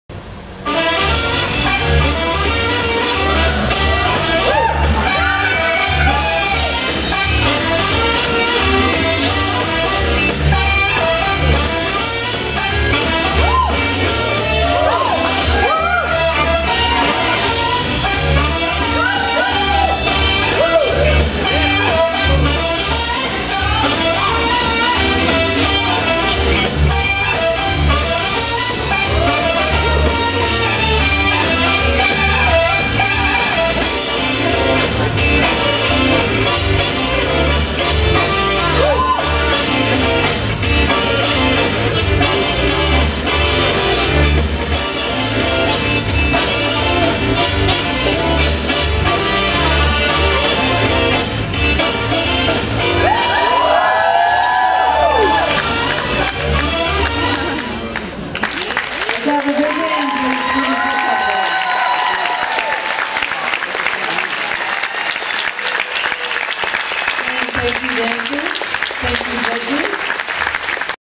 Jive (Zsolt & Timea, 2008-11-08)
Zsolt Katona and Timea Potys dance jive in the Adult (19-34) Championship Latin (CSRPJ) division at the 2008 USA Dance California State Dancesport Championships.

(At Just Dance Ballroom, Oakland, CA on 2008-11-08)